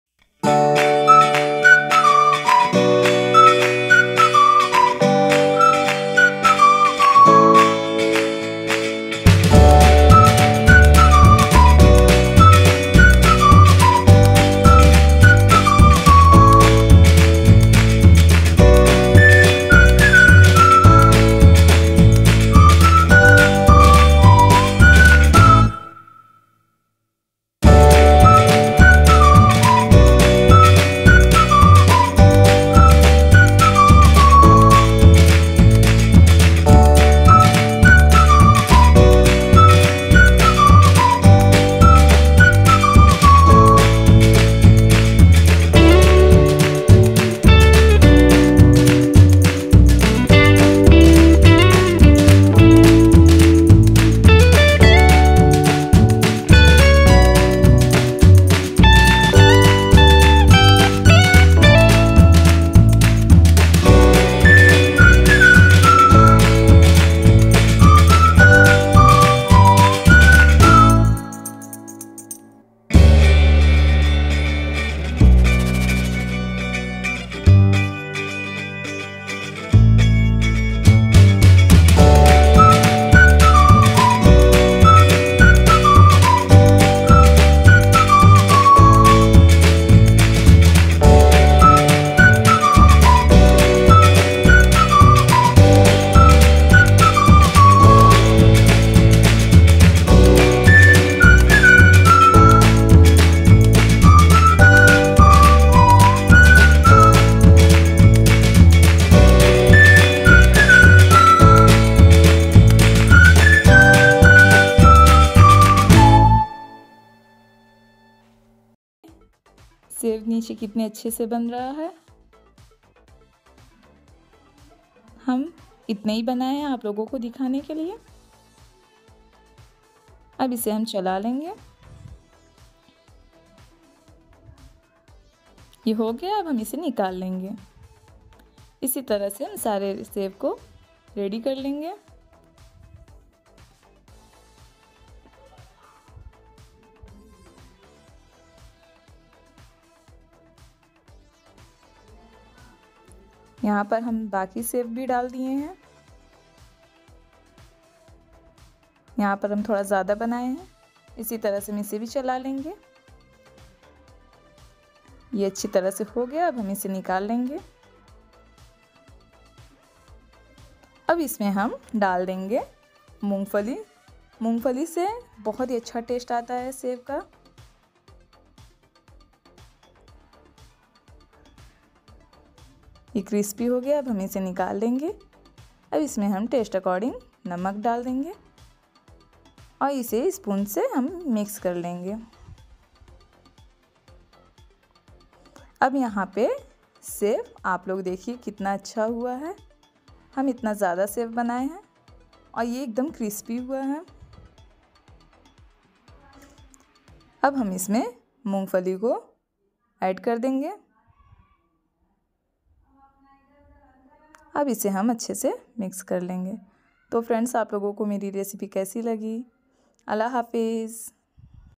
Homemade Besan Ki Sev Recipe| Crispy Sev Namkin -EASY Sev Recipe
Assalamualaikum

Hi Friends presenting you a simple and Crispy Besan Sev which can be used in any Namkin. This Sev is an all purpose Sev .do try this Sev recipe at home 🏠

Ingredients:-

1/2 kg Besan
1tsp salt
1/2 tsp turmeric
100gm peanut
2tsp oil

Instagram.com/iramcookwith